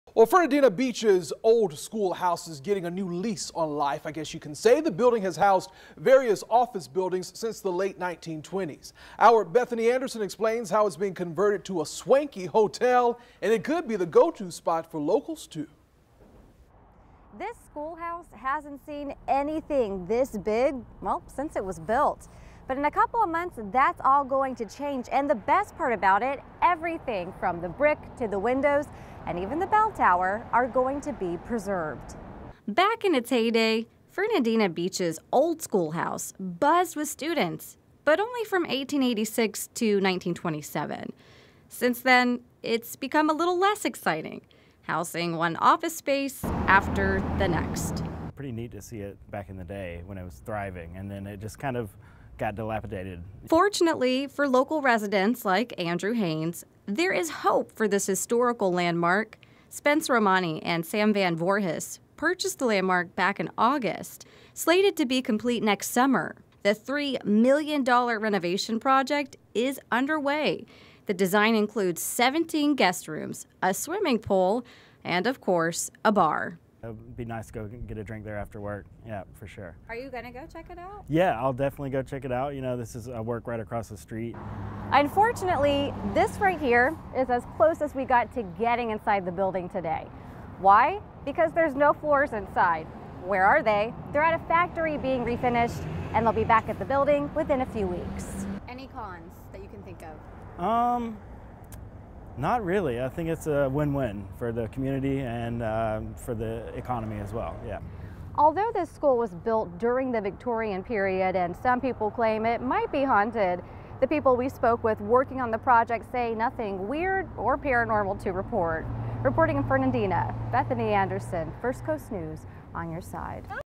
Fernandina Beach school house turning into hotel
The Old School House built in the 1920s is getting a makeover and being converted into a swanky hotel.